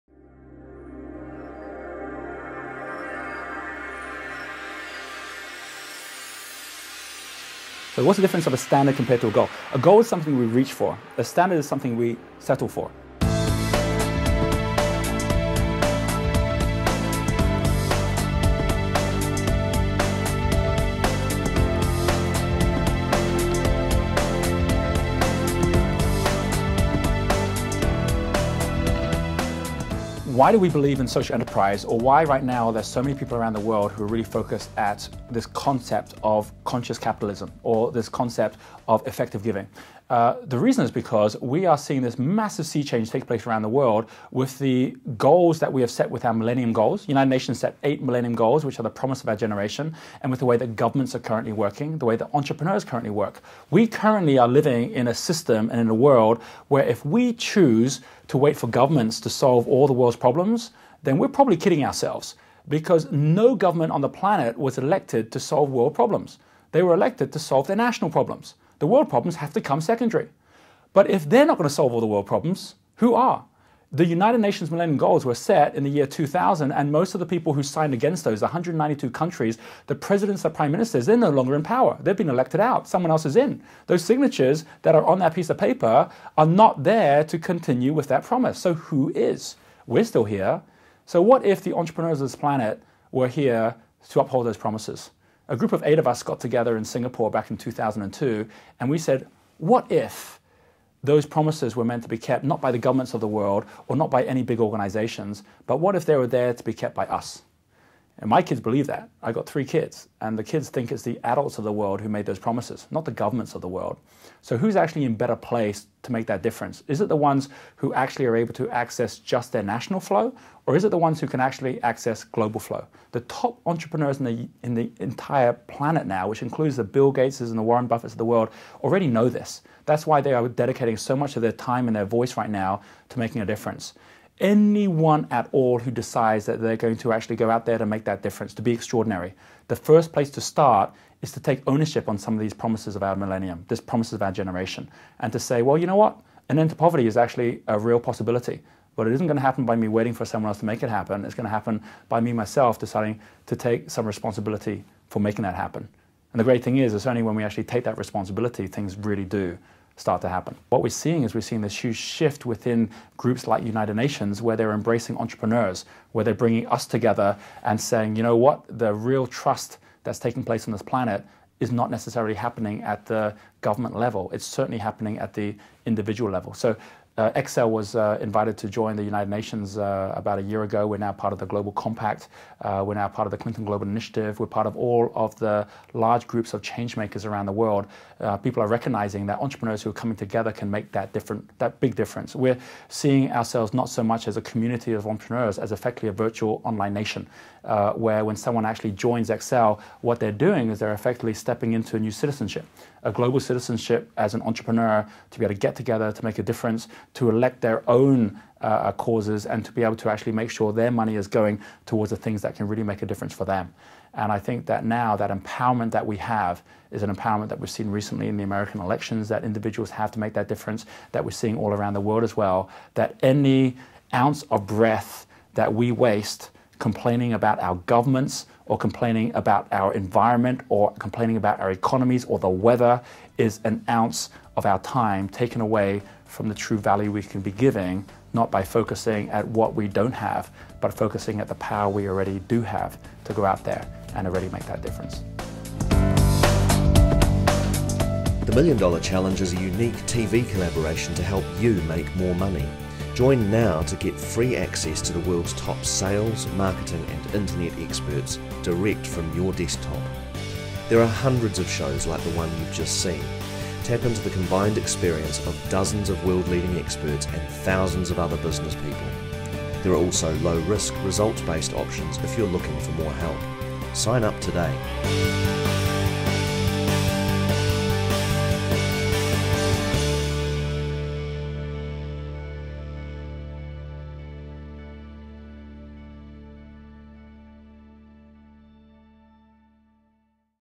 Secrets to success and conscious Capitalism....more business development advice from Roger Hamilton
Secrets to success and business development advice In this new business television show, Wealth Dynamics founder Roger Hamilton asks whether conscious capitalism can address world problems where national governments have failed?